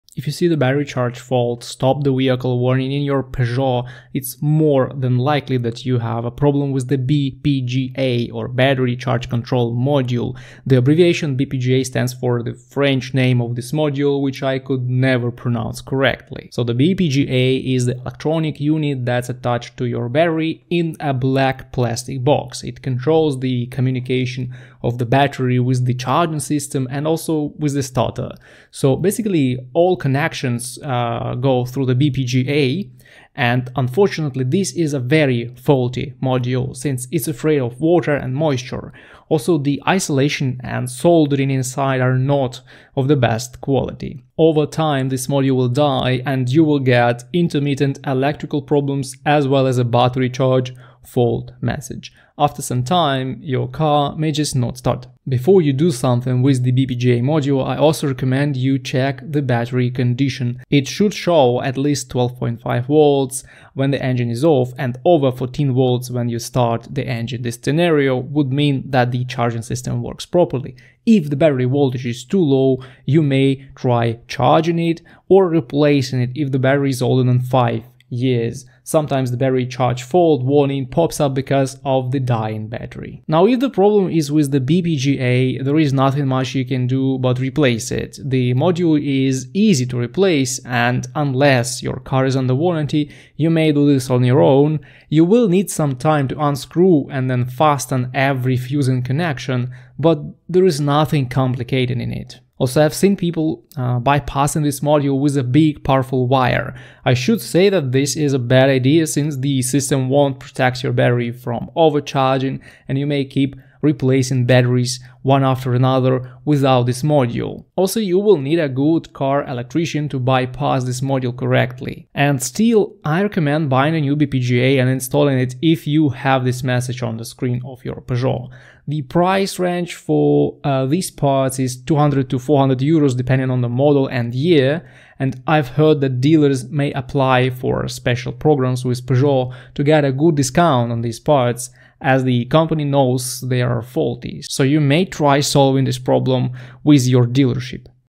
“Battery charge fault, stop the vehicle” Warning in Peugeot
If you see the message saying “Battery charge fault, stop the vehicle” in your Peugeot vehicle, there are two possible culprits: the dying battery (less common) and the BPGA module. The latter one controls the way your 12V battery is getting charging voltage. And sometimes it can just cut off the battery from the electrical system in your car.

Unfortunately, this is not a cheap module and in this video, I will tell you how you can solve this problem on your own or with the dealership and pay less money in the end.

You should know that BPGA problems are extremely common for certain model years of Peugeot vehicles. But unfortunately, I found no TSBs or recalls for this. And still, I know about cases when people got good discounts for parts when they went directly to dealerships with this problem. This is a very well-known issue.